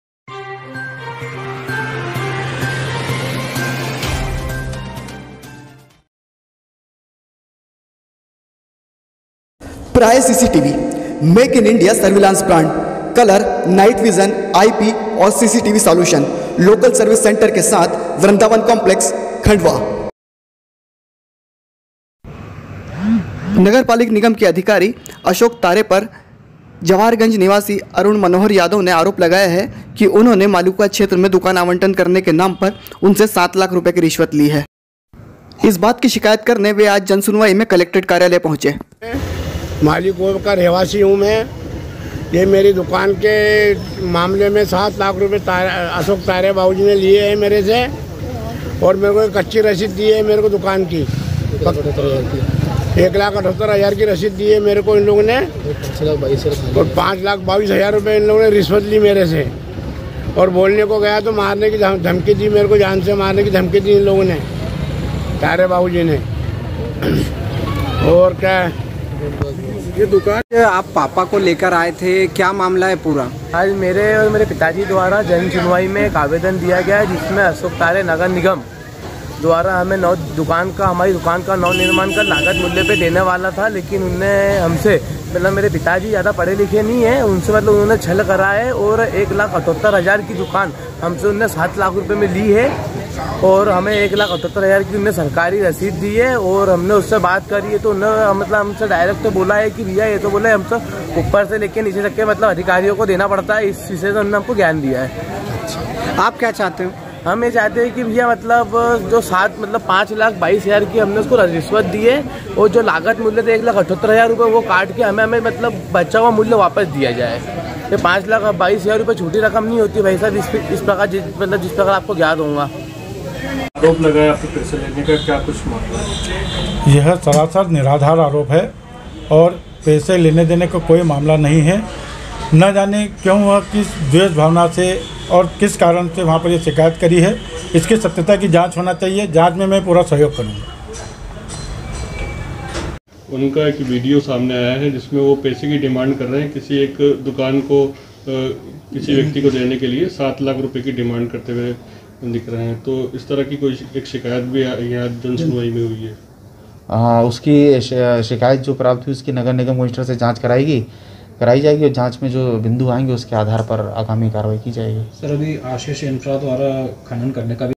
Khandwa-निगम अधिकारी पर अरुण यादव ने सात लाख की रिश्वत का लगाया आरोप,अधिकारी का कहना फसाने की कोशिश
Khandwa-निगम अधिकारी पर अरुण यादव ने सात लाख की रिश्वत का लगाया आरोप,शिकायत लेकर जनसुनवाई में कलेक्टर कार्यालय पहुंचे।अधिकारी का कहना फसाने की कोशिश की जा रही है।@NewsfirstKnw